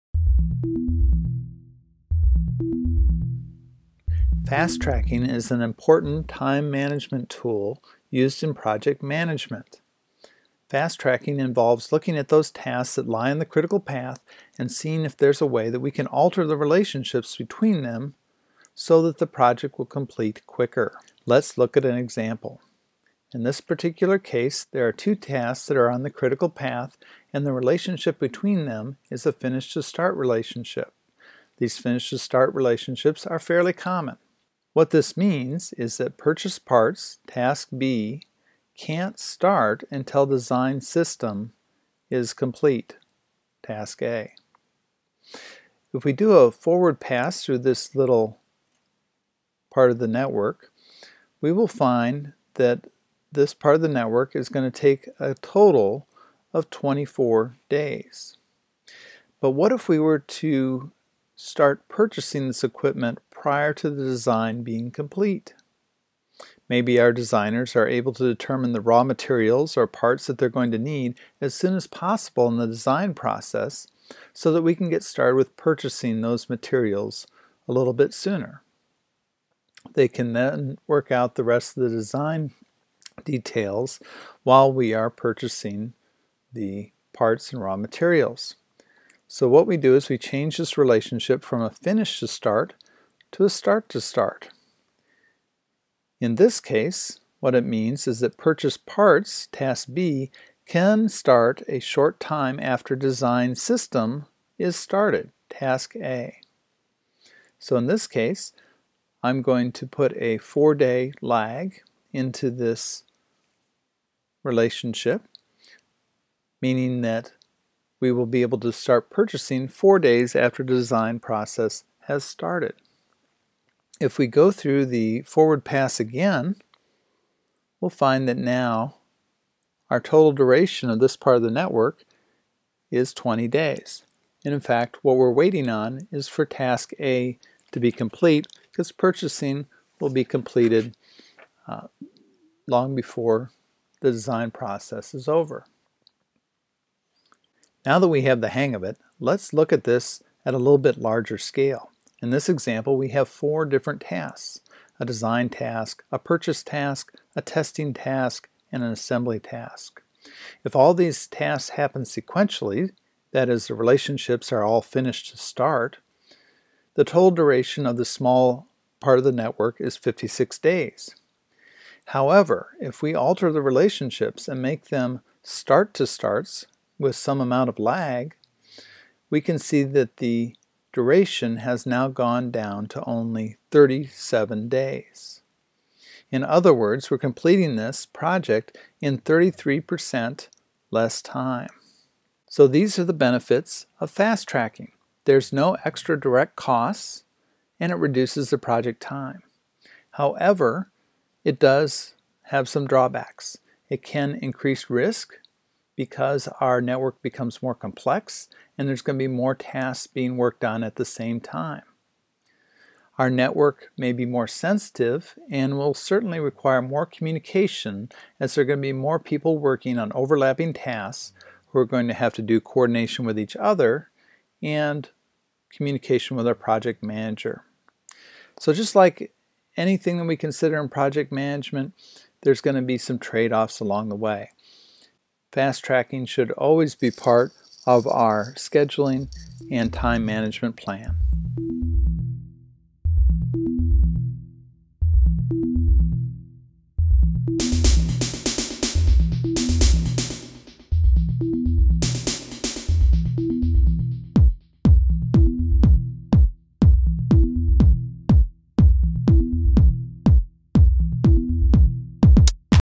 Project Time Compression: Fast Tracking
ast Tracking is a common tool used in project management. This short video introduces the fundamentals of fast-tracking as well as some of the downsides.